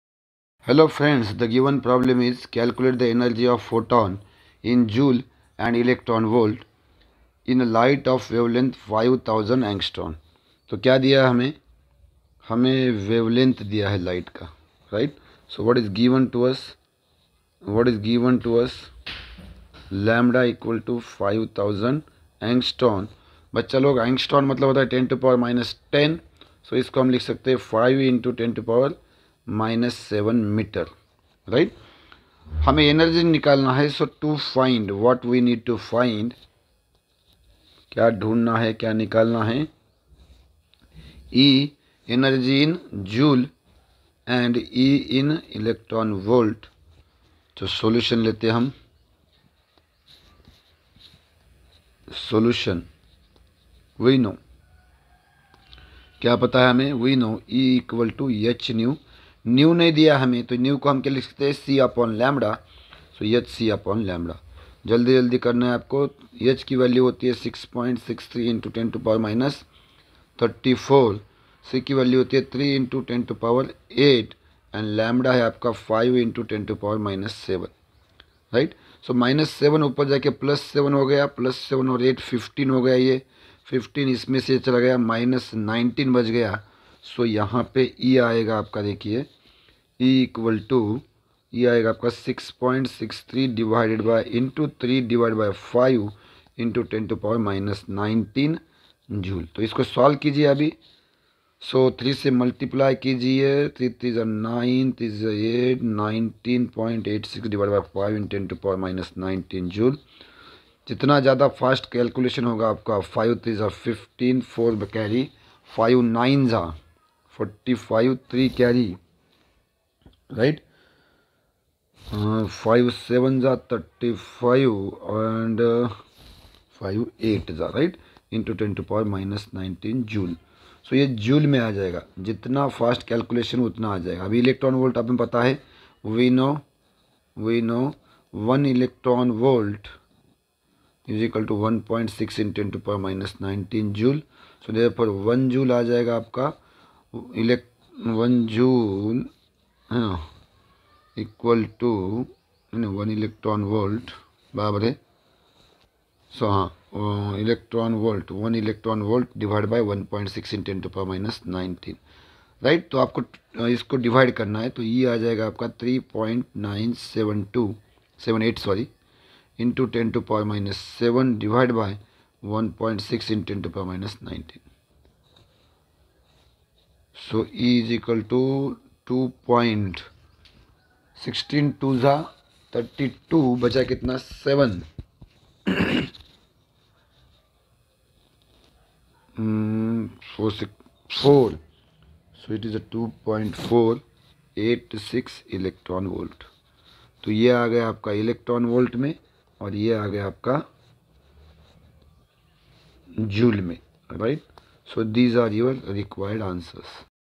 Dual nature-Calculate the energy of a photon, in joule and eV,_ in a light of wavelength 5000 Å.
Dual nature-Calculate the energy of a photon, in joule and eV,_ in a light of wavelength 5000 Å. (2 marks)